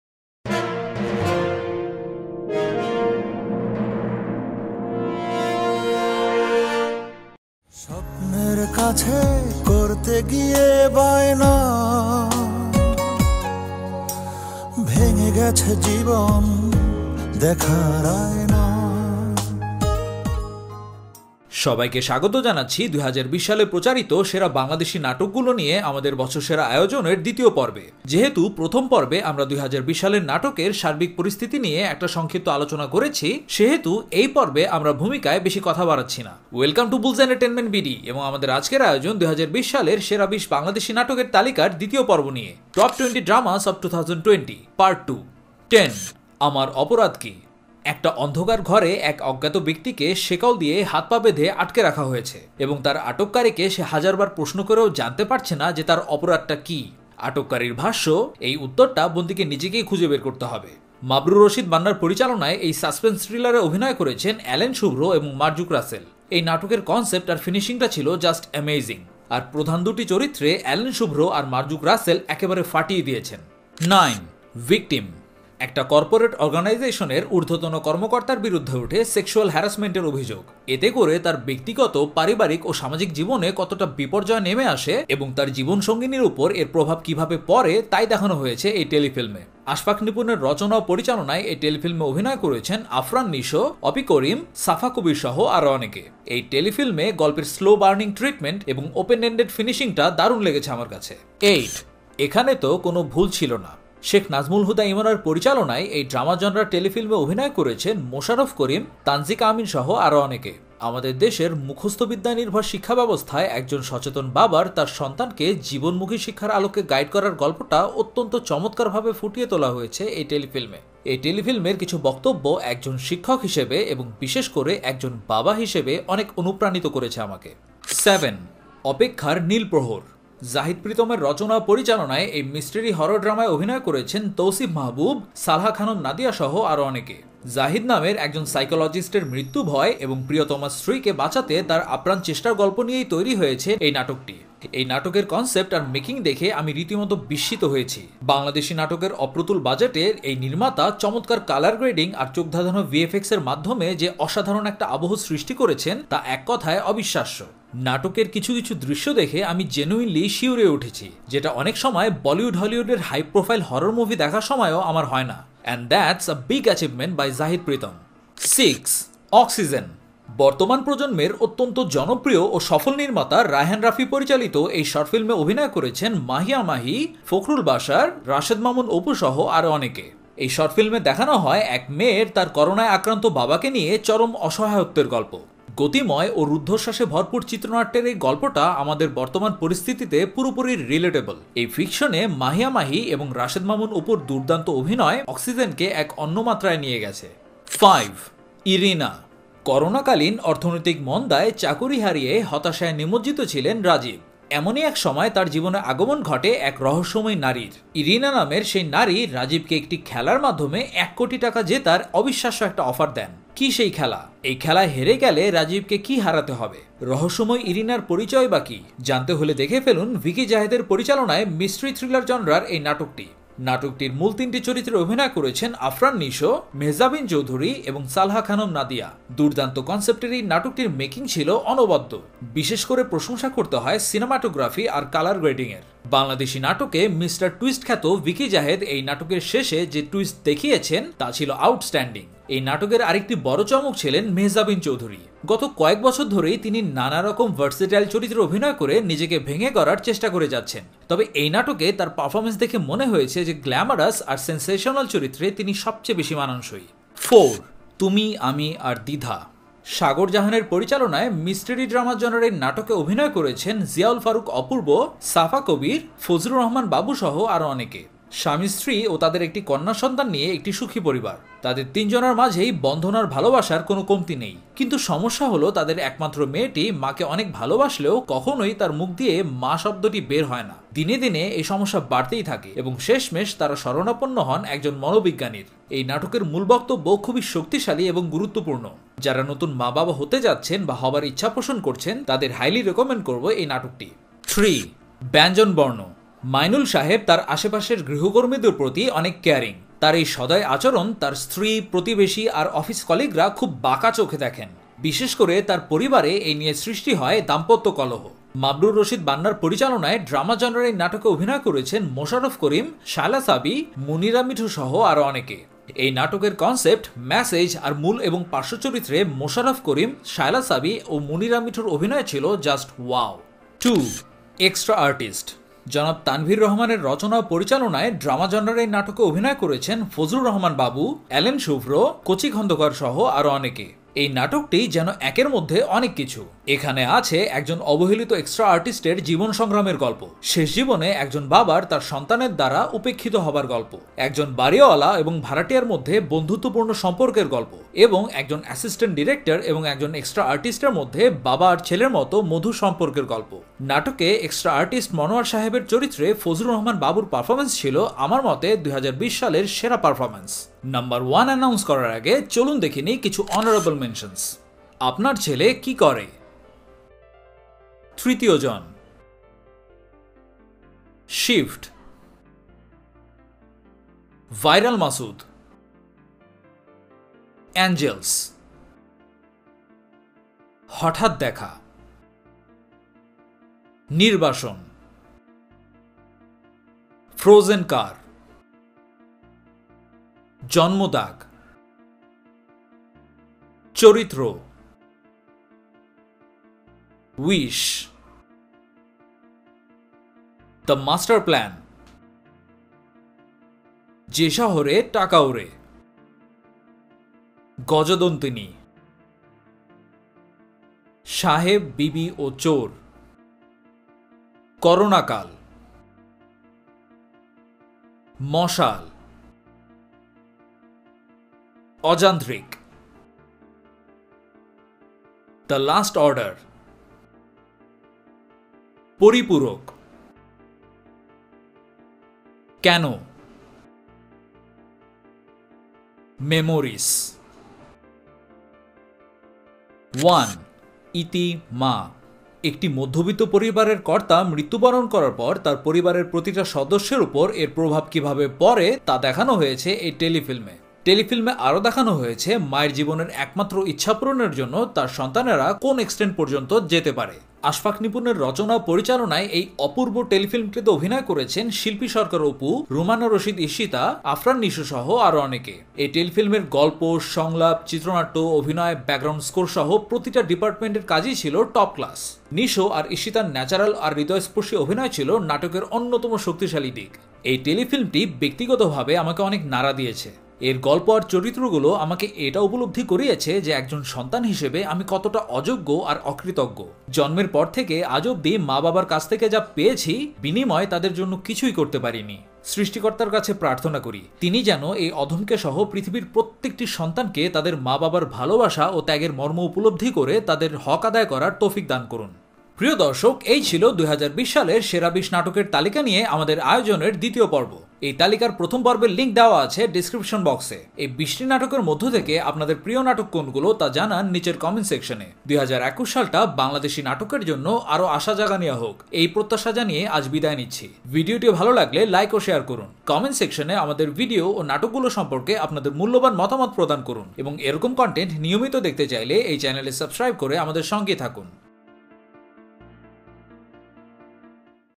সেরা ২০ নাটক-২০২০ঃ পর্ব ২ ।। Top 20 Dramas of 2020: Part 2 ।। Bangladeshi Natok 2020
দেখে নিন ২০২০ সালের সেরা ২০ নাটক নিয়ে আমাদের টপচার্টের দ্বিতীয় পর্ব।